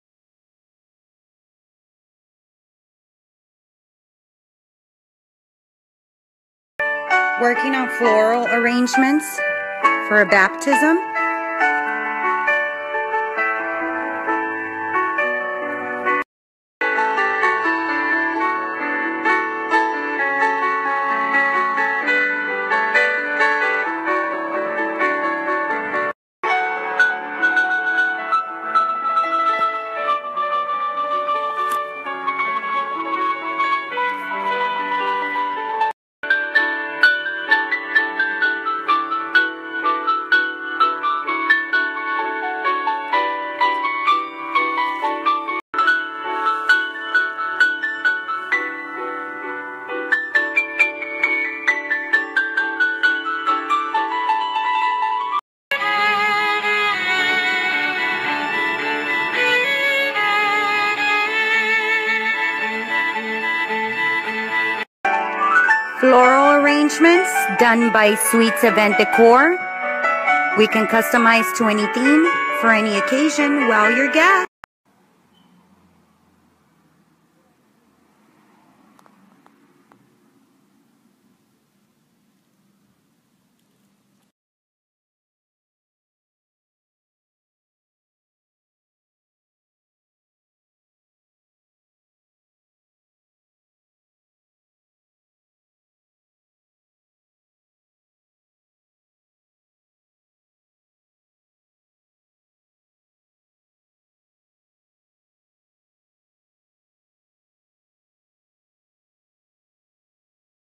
Floral Arrangements by: Sweets Event Decor
Natural Floral Arrangements Event Decor by: Sweets Event  Decor

Bring your events to life! We can customize to any theme for any occasion. Book your event today & WOW your guests!!!

Servicing the Los Angeles & surrounding areas.